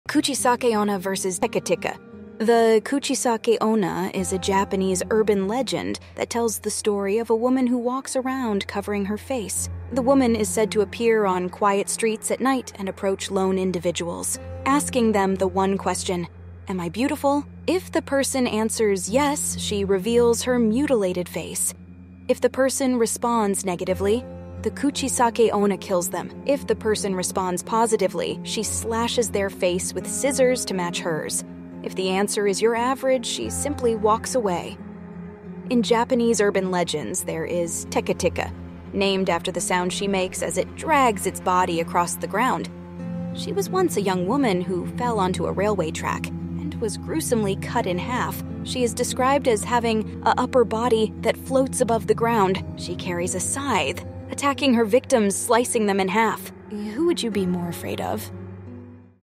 Kuchisake-Onna vs. Teke Teke: Which Japanese Urban Legend is Scarier?
Explore two terrifying Japanese urban legends, Kuchisake-Onna and Teke Teke, and decide which one is more spine-chilling. Kuchisake-Onna is a mutilated woman who slashes the faces of those who answer her question wrong, while Teke Teke is a vengeful spirit who drags herself along the ground, slicing her victims in half. Watch this video to learn more about these gruesome legends and find out which one will haunt your nightmares.